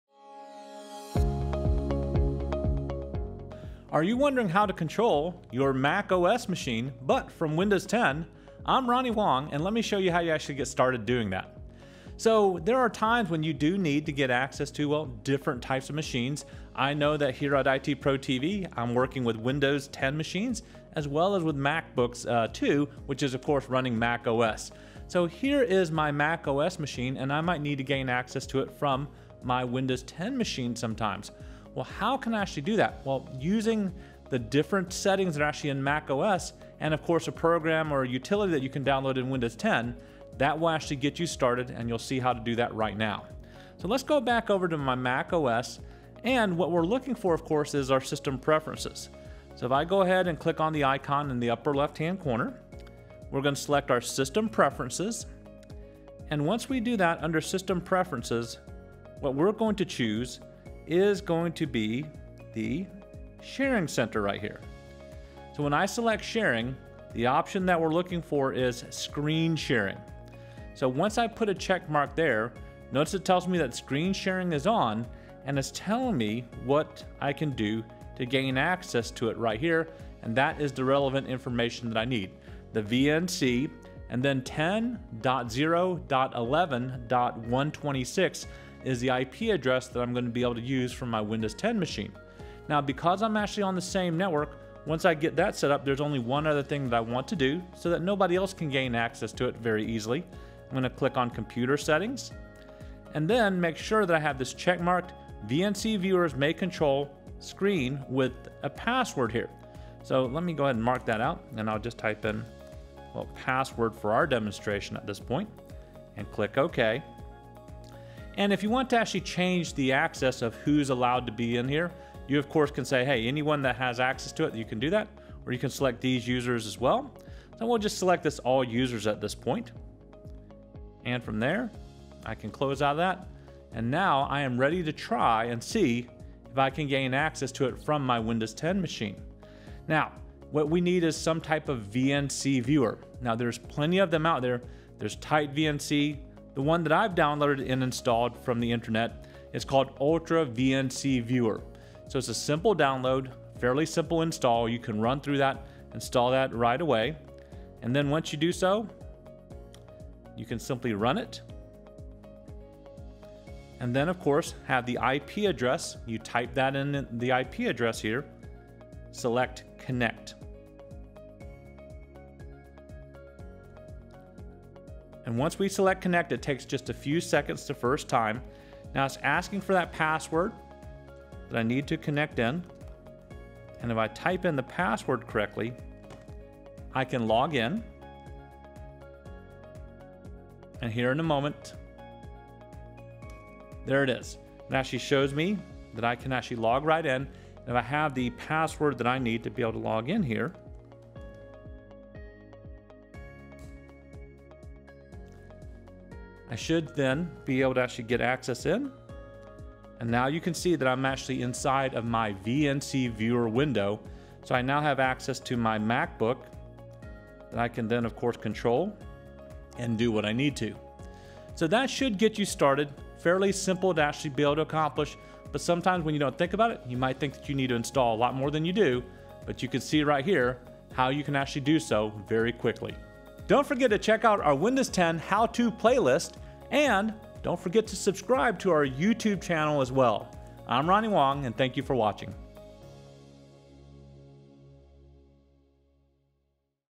How to Easily Remote Access your Mac from Windows 10 with VNC client
Are you wondering how you can control or remote access your Mac machine from a Windows 10 PC?  In this video, Ronnie will show you how to remote desktop easily using Screen Sharing on macOS and a VNC client on Windows 10.